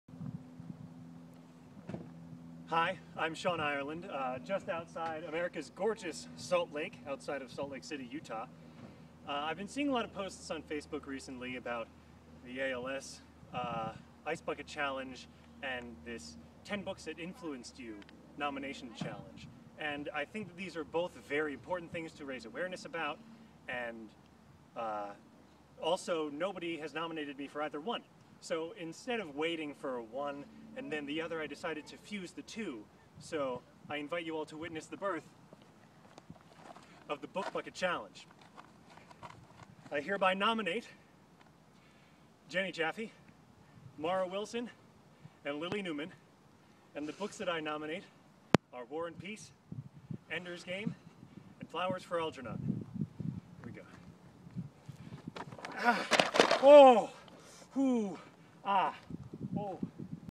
Sean Ireland Bucket Challenge
I filmed this while I was in Salt Lake City. It was dated then, and has only become more so. But I thought it was time I chimed in.

There are a lot of great organizations that could use your donations! My favorites are:

Thanks!